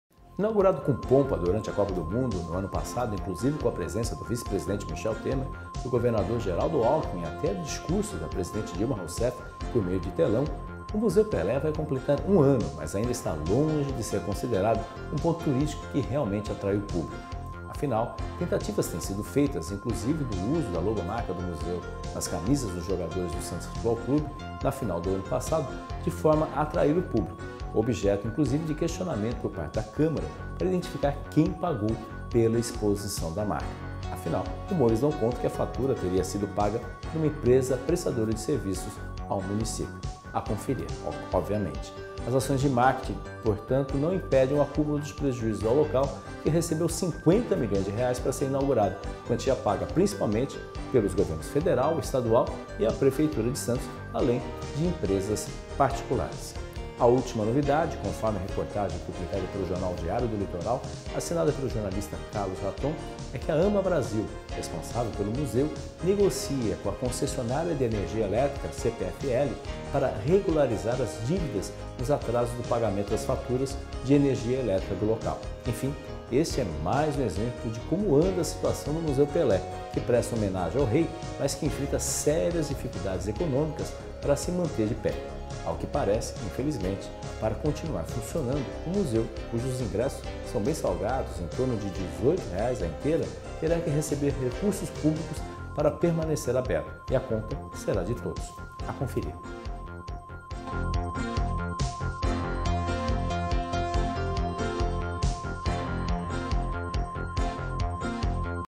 BoqnewsTV - Museu Pelé
Inaugurado com pompa durante a Copa do Mundo, o Museu Pelé coleciona sucessivos prejuízos, o que tem comprometido seu futuro. A bola da vez é a negociação entre a responsável pelo museu, Ama Brasil, e a CPFL, concessionária de energia em razão de atrasos nos pagamentos, conforme reportagem divulgada na Imprensa. Confira a análise do jornalista Fernando De Maria na BoqnewsTV.